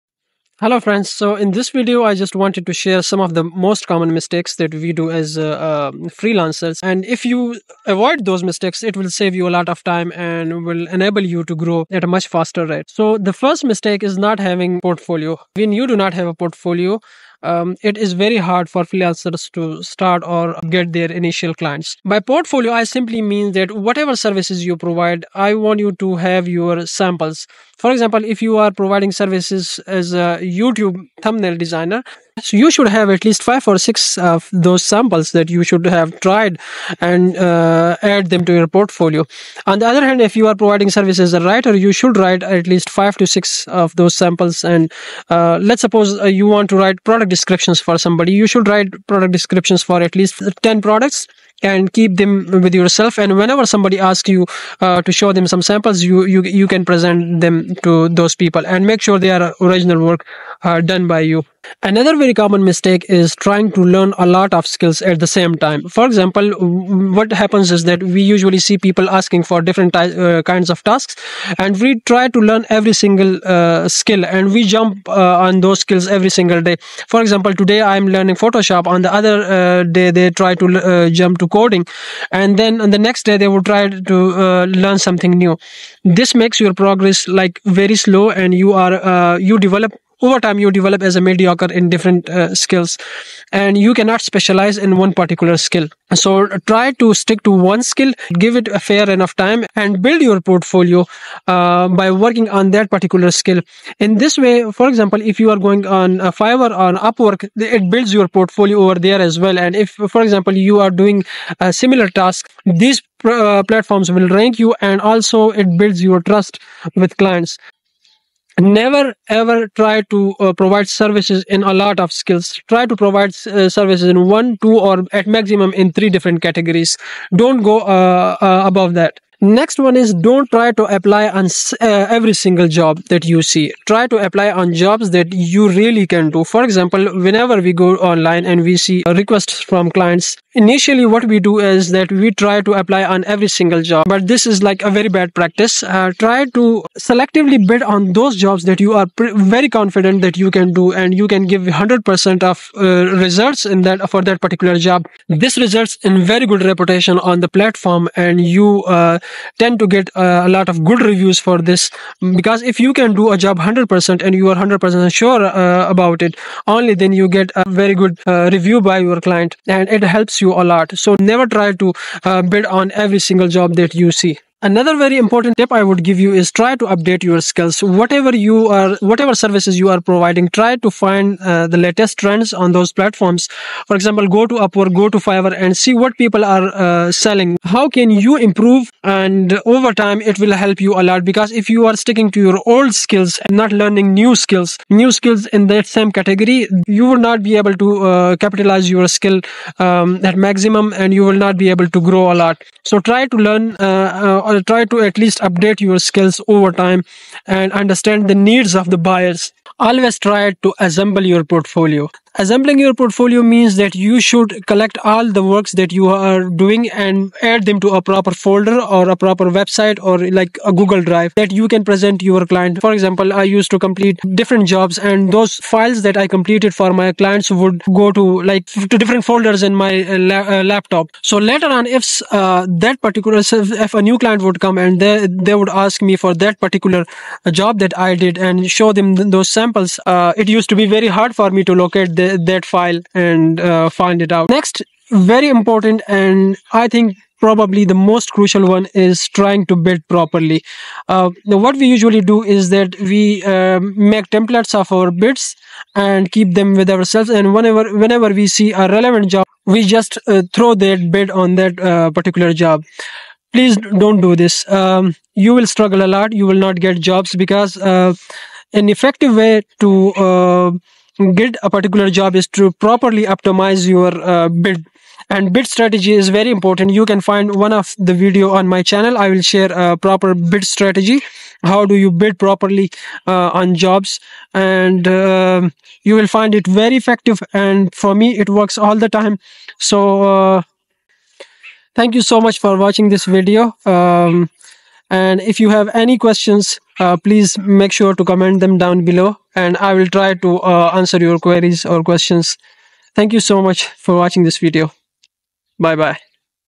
Deadly Mistakes Freelancers Make | Never do These Mistakes as a Freelancer
Most Freelancers usually start and give 100% of their efforts but they fail because of some of the most common mistakes they do in their freelance journey.

In this video, I have explained most of these mistakes and the main topics I cover is a discussion on;
1. Portfolio
2. No of Skills and Categories
3. Bid Proposals
4. Applying on Jobs Request etc.

I have also explained different mistakes with examples.

Make sure to watch this video till the end for maximum benefit.

Regards
Nabi Amin